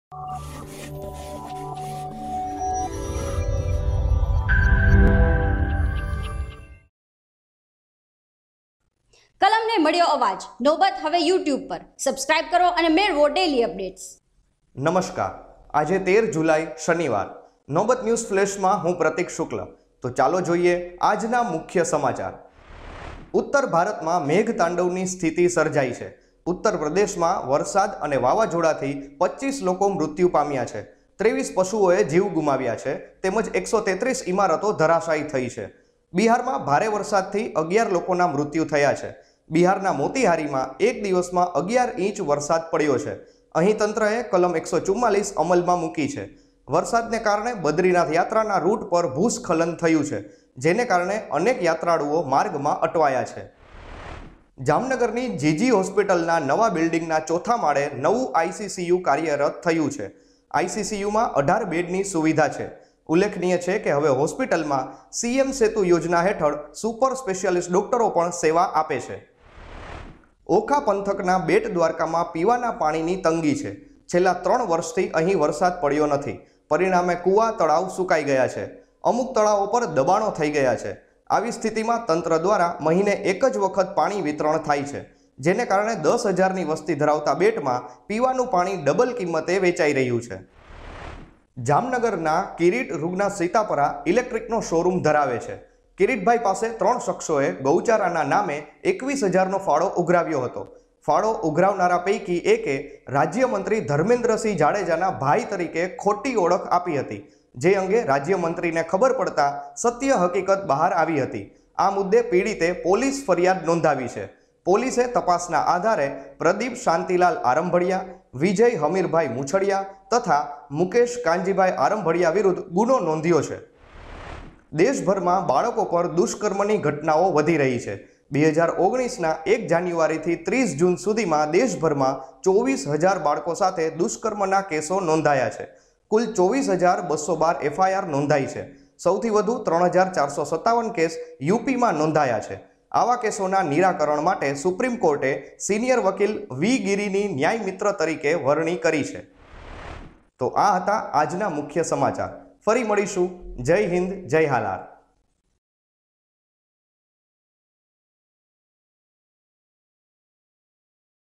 News Flash: 26 people died due to heavy rain & thunderstorm in northern India
1. 26 people died due to heavy rain & thunderstorm in northern india.
2. Jamnagar: 18 bed ICCU started in GG Hospital.
3. People suffering from water crisis in okha’s bet dwarka.
4.  Rs 21,000 fraud on Jamnagar’s businessman in the name of cow wellness fund.
5. 24,000 children have been raped in India till June - 2019.

Image Source: Internet